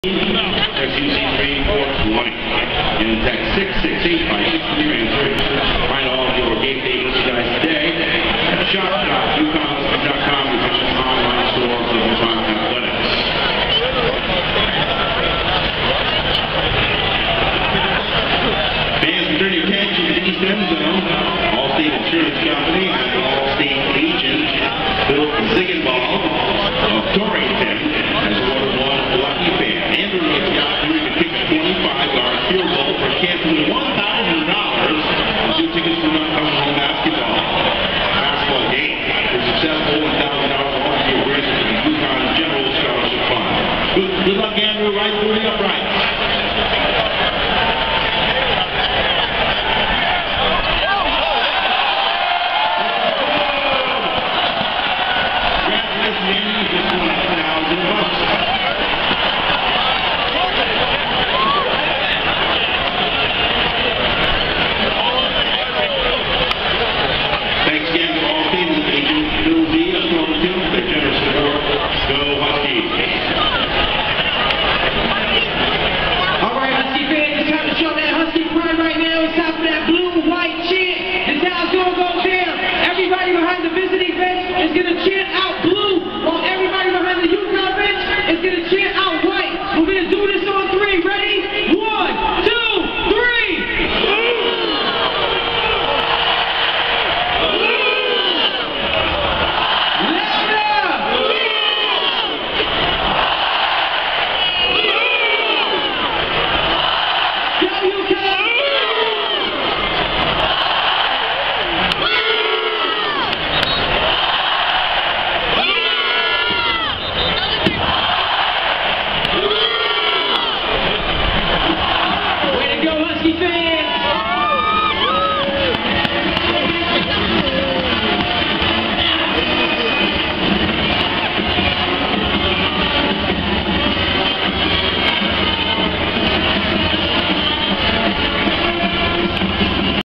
Field Goal contestant wins at Cincinnati vs UConn football game - 12-01-12
Field Goal contestant wins at Cincinnati vs UConn football game
Rentschler Stadium  -  East Hartford, Connecticut  -  Saturday December 01, 2012